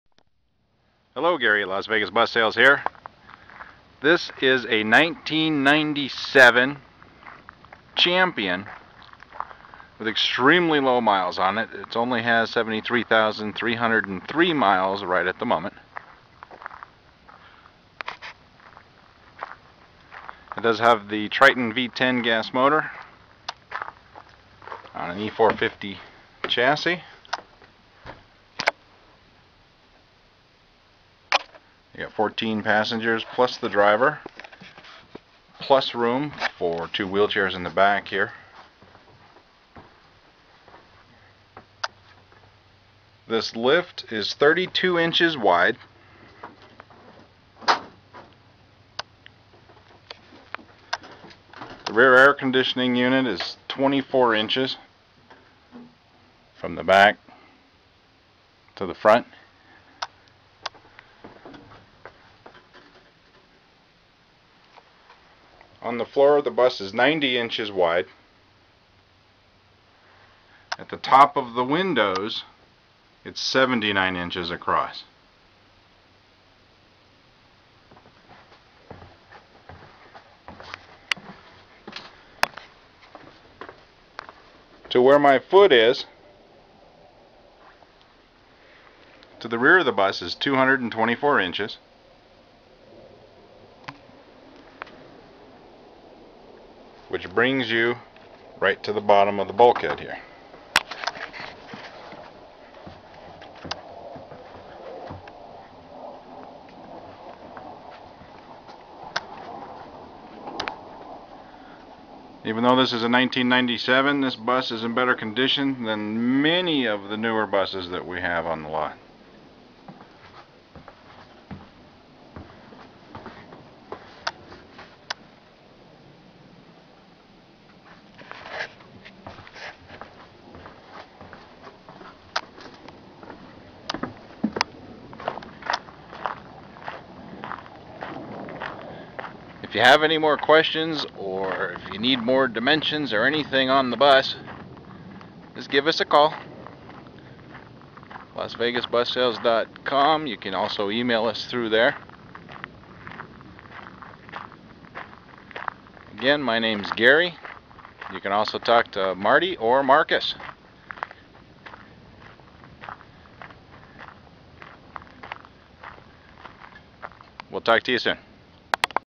One of our Awesome used buses for sale 1997 Champion 14 Passenger and 2 wheelchairs
This is one of our lowest mileage used buses for sale. see all of our buses and the detail page for this used bus is